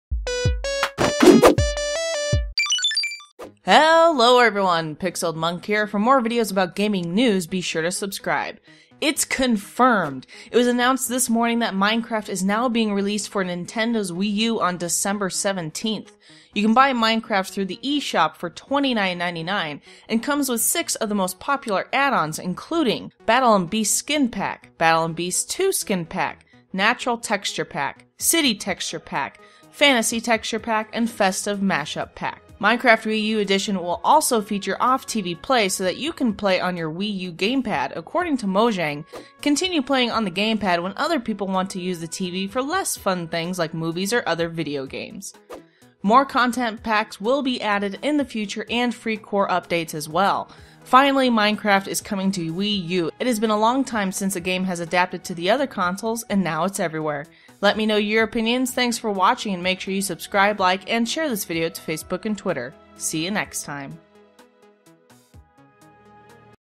Confirmed! Minecraft Released For Wii U On December 17th (Gaming News)
Mojang has announced that Minecraft will be coming to Wii U on December 17th.